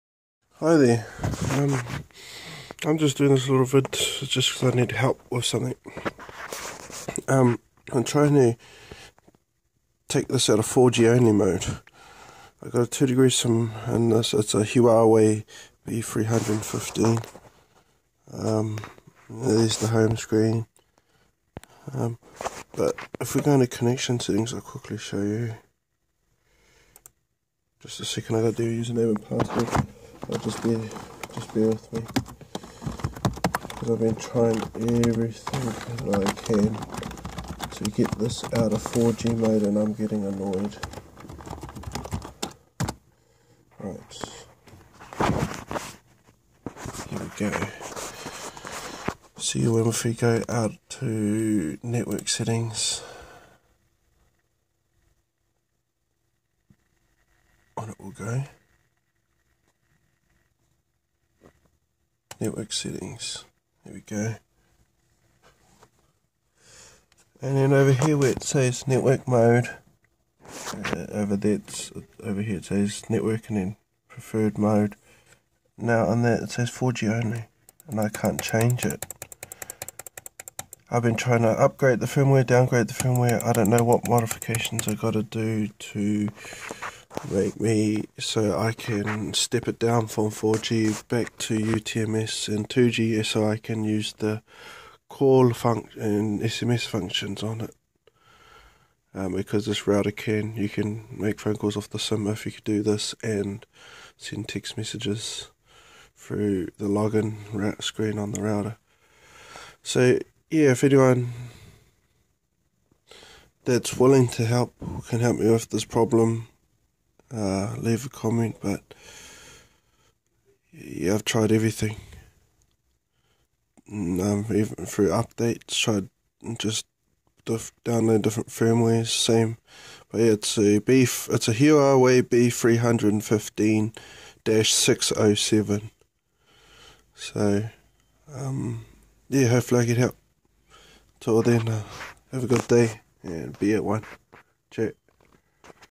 How do i upgrade or downgrade huawei b315-607 so i can use UTMS and GSM networks
I am trying to changed the preffered mobile networks to 2G and 3G. If i do that i can make phone calls and send text messages. Chur